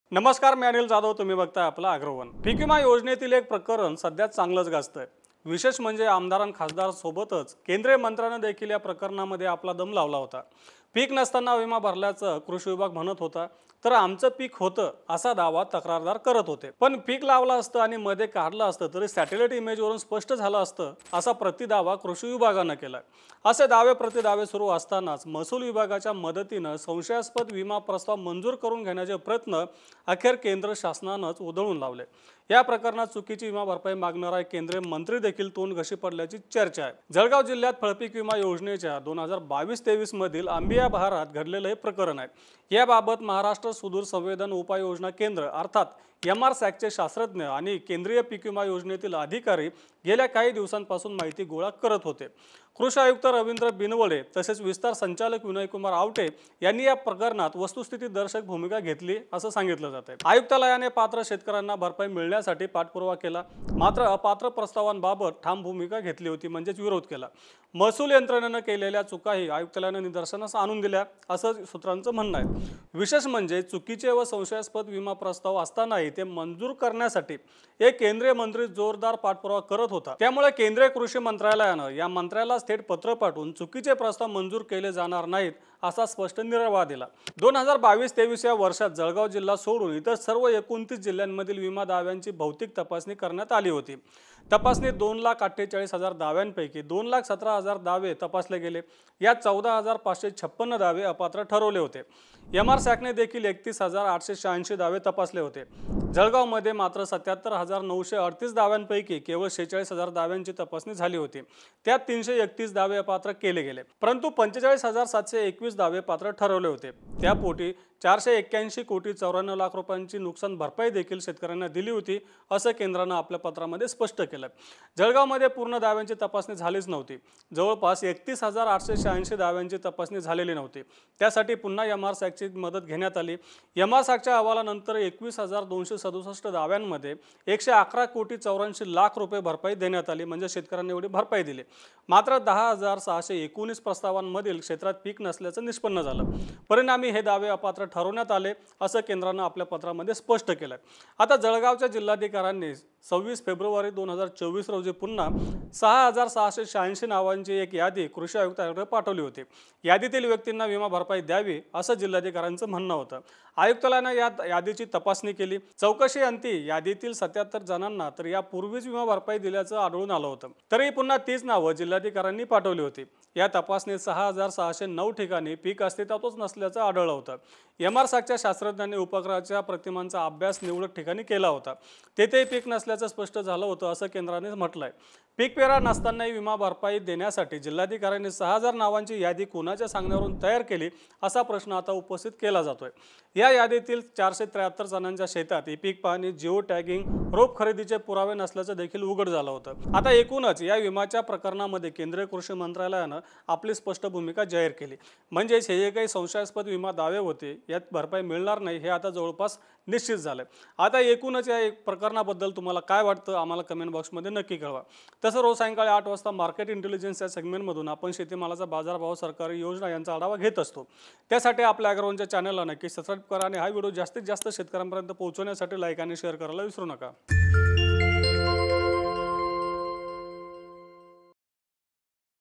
Banana Crop Insurance : राज्यातील एका केंद्रीय मंत्र्यालाही फटकारले; केळीचे संशयास्पद दावे फेटाळले
पीकविमा योजनेतील एक प्रकरण चांगलच गाजत आहे. विशेष म्हणजे आमदार, खासदारांसोबतच केंद्रीय मंत्रीही या प्रकरणात आपला दम लावत होते. पीक नसताना विमा भरल्याचे कृषी विभाग म्हणत होता तर आमचं पीक होतं, असा दावा तक्रारदार करत होते. पीक लावलं जरी असतं तर सॅटेलाईट इमेजवरून स्पष्ट झाले असते, असा प्रतिदावा कृषी विभागाने केला.

A case in the crop insurance scheme is well known. Interestingly, along with MLAs, MPs, Union Ministers were also putting their efforts in this matter. The complainant was claiming that the agriculture department was saying that the insurance was paid when there was no crop, but we had a crop. The agriculture department countered that even if the crop had been planted, it would have been clear from the satellite image.